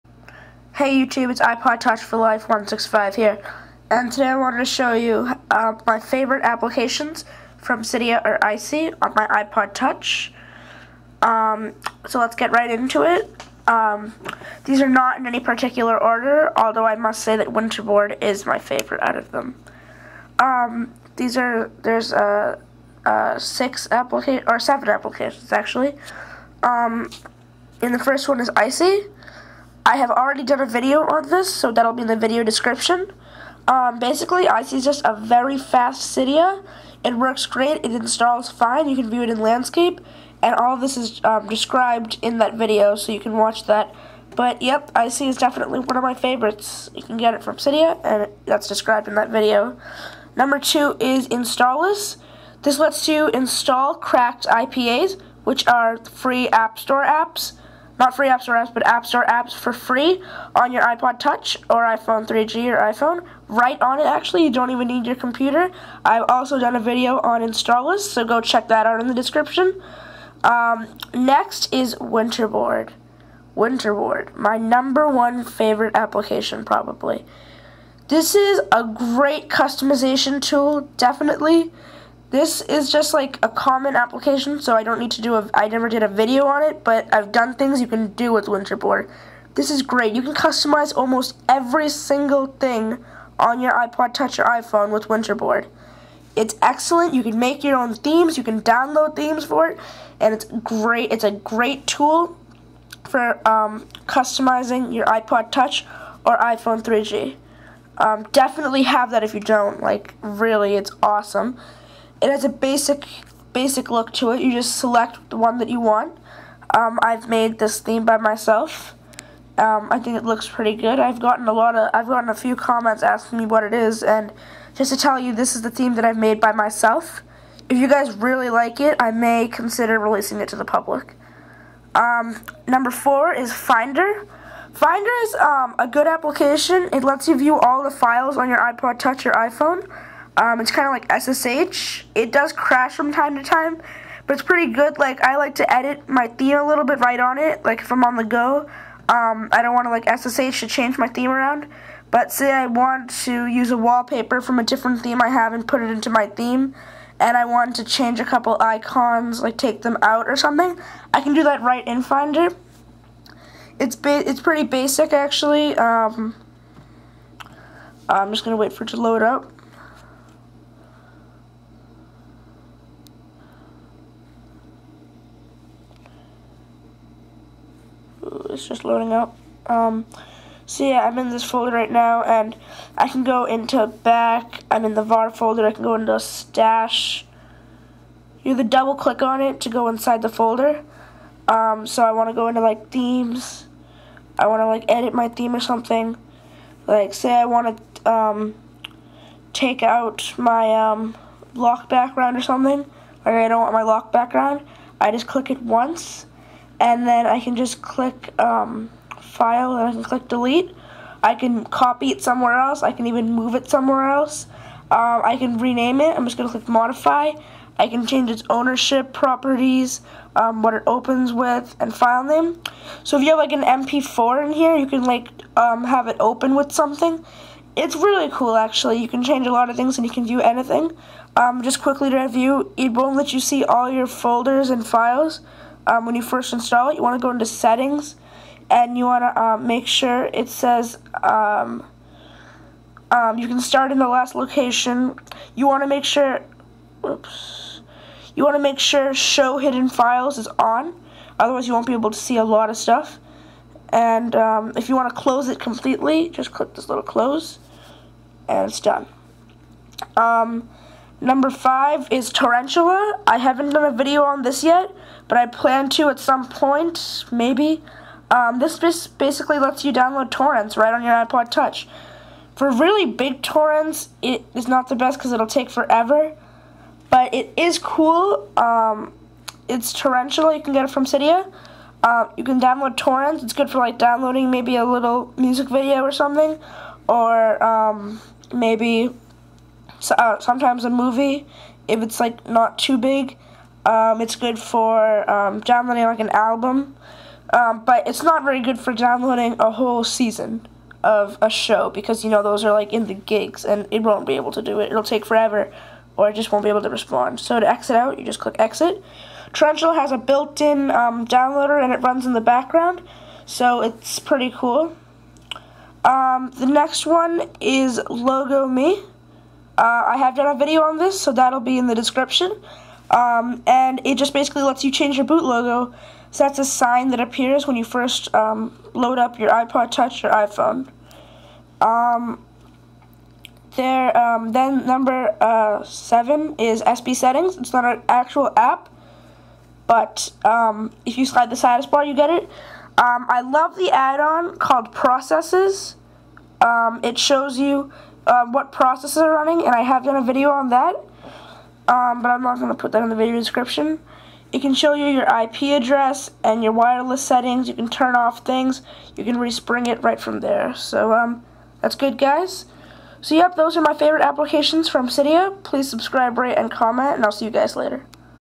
My Favorite Apps From Cydia: iPod Touch and iPhone
README!!!

Hey guys.  Today I wanted to who you my favorite Cydia/Icy applications.  They go in no particular order

3. Winterboard

4. Finder

5. Torrentula

7. SBSettings

BONUS: Packages form Cydia that aren't applications:

1. Safari DL Plugin